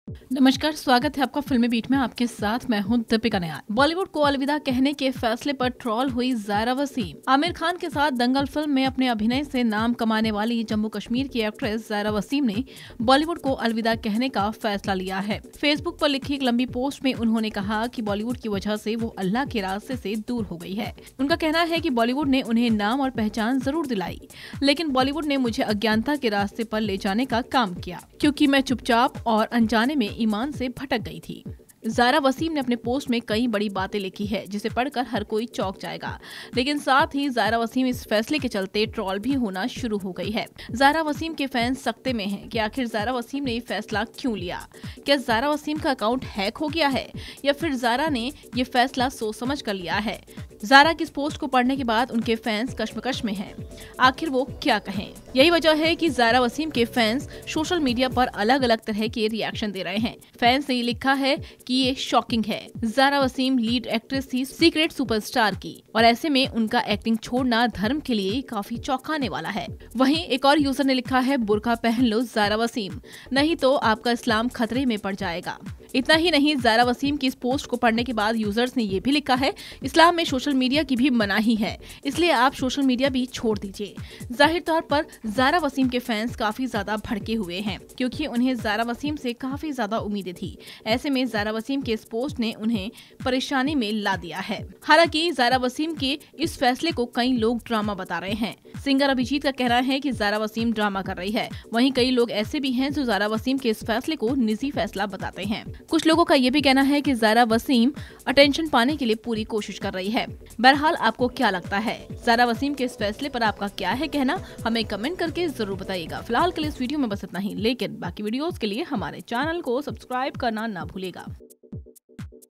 Zaira Wasim controversy: fans lashes out at her for leaving Bollywood| FilmiBeat
Zaira Wasim fans lashes out at her for leaving Bollywood. In a long post shared on her Facebook, Twitter and Instagram accounts, the 18-year-old wrote, "I started to become the prime candidate of public attention, I was projected as the gospel of the idea of success and was often identified as a role model for the youth. However, that's never something that I set out to do or become, especially with regards to my ideas of success and failure, which I had just started to explore and understand." Watch this video to know more!